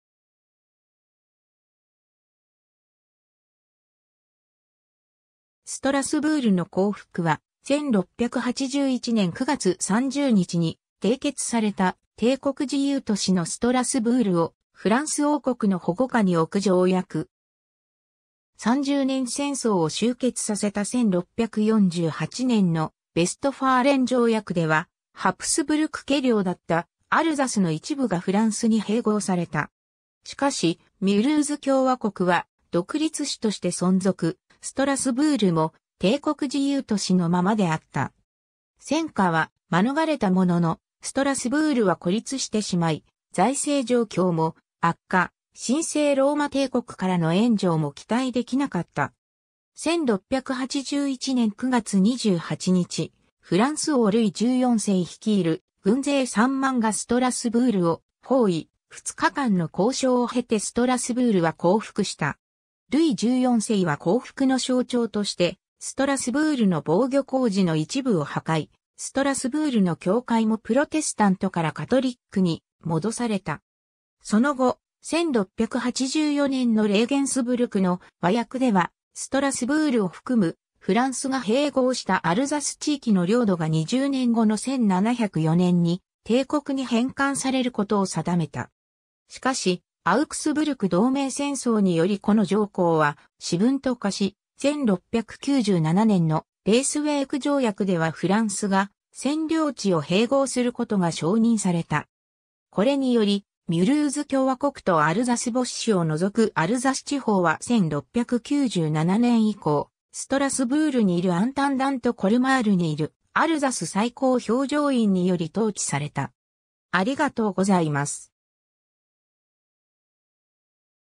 ストラスブールの降伏
ストラスブールの降伏（ストラスブールのこうふく、フランス語: Capitulation de Strasbourg）は、1681年9月30日に締結された、帝国自由都市のストラスブールをフランス王国の保護下に置く条約。
 三十年戦争を終結させた1648年のヴェストファーレン条約ではハプスブルク家領だったアルザスの一部（主に南部）がフランスに併合された。
しかし、ミュルーズ共和国は独立市として存続、ストラスブールも帝国自由都市のままであった。
戦禍は免れたものの、ストラスブールは孤立してしまい、財政状況も悪化、神聖ローマ帝国からの援助も期待できなかった。
 1681年9月28日、フランス王ルイ14世率いる軍勢3万がストラスブールを包囲、2日間の交渉を経てストラスブールは降伏した。
ルイ14世は降伏の象徴としてストラスブールの防御工事の一部を破壊、ストラスブールの教会もプロテスタントからカトリックに戻された。
 その後、1684年のレーゲンスブルクの和約ではストラスブールを含む、フランスが併合したアルザス地域の領土が20年後の1704年に帝国に返還されることを定めた。
しかし、アウクスブルク同盟戦争によりこの条項は死文と化し、1697年のレイスウェイク条約ではフランスが占領地を併合することが承認された。
これにより、ミュルーズ共和国とアルザス・ボッシュを除くアルザス地方は1697年以降ストラスブールにいるアンタンダンとコルマールにいるアルザス最高評定院により統治された。